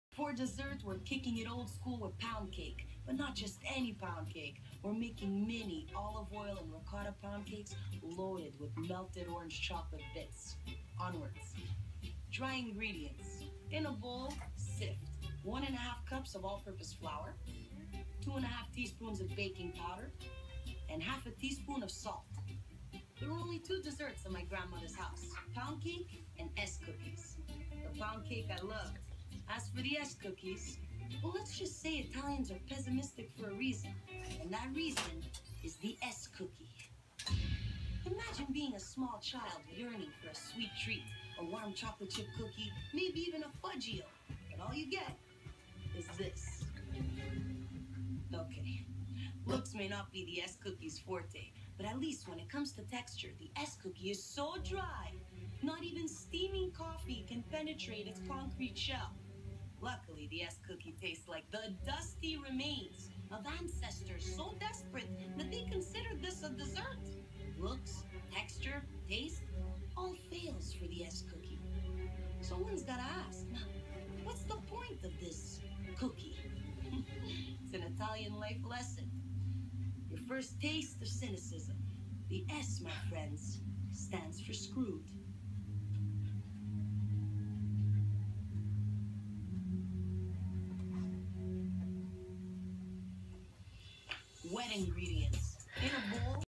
Nadia G explains the S cookie
Great description by Nadia G of every child that grew up in an Italian family with the S cookie.